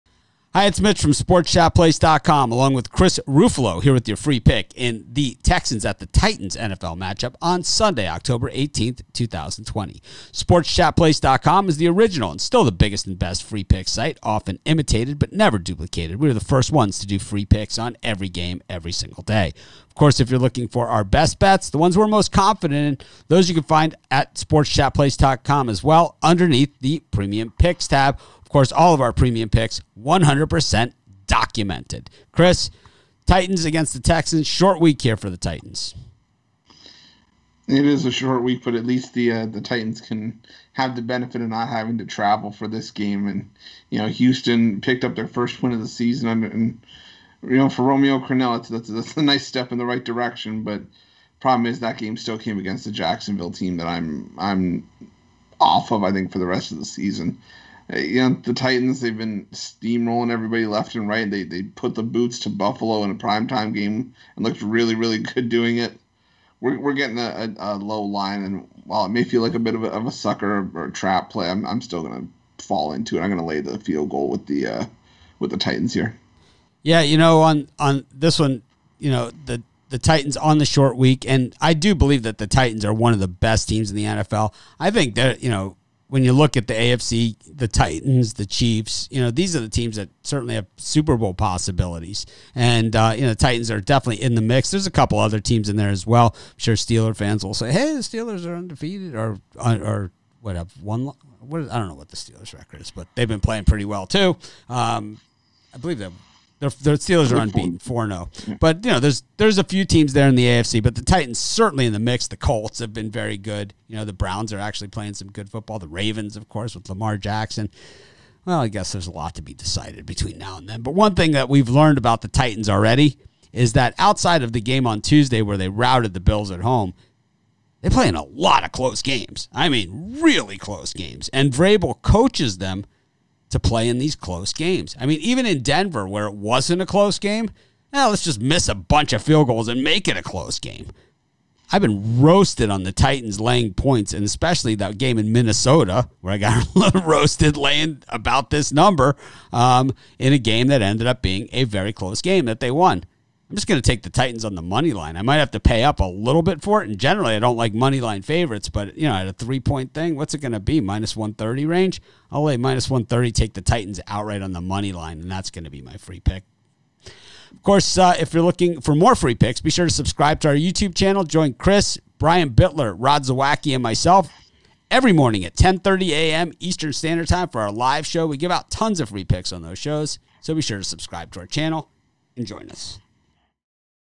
Houston Texans at Tennessee Titans Sunday 10/18/20 NFL Picks & Predictions Week 6
The NFL is in Week 6 and we have all your NFL betting needs. Our free betting pick is on the Houston Texans at Tennessee Titans matchup on Sunday October 18, 2020 and we have your Free expert Betting Picks from Sports Chat Place experts Mitch Wilson and Chris Ruffolo.

Get the hottest sports betting picks, odds, and predictions daily. It's about all Sports Chat Place as we put the money in your pocket.